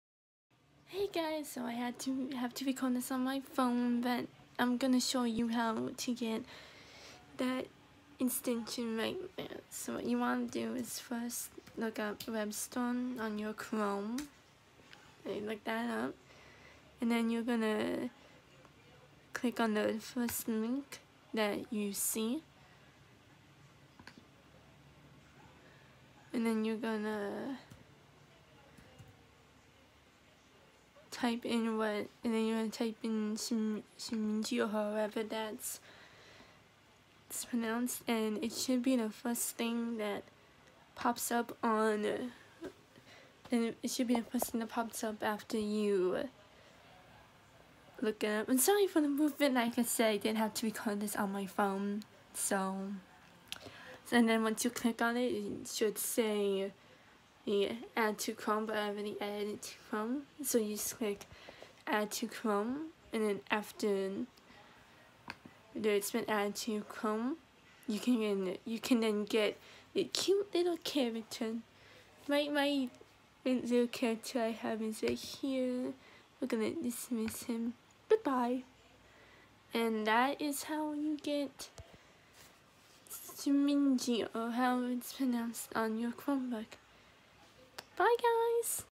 How to get Shimeji on your Chromebook
Hello and in this video I show you how to get Shimeji on your Chromebook. Bye guys ^^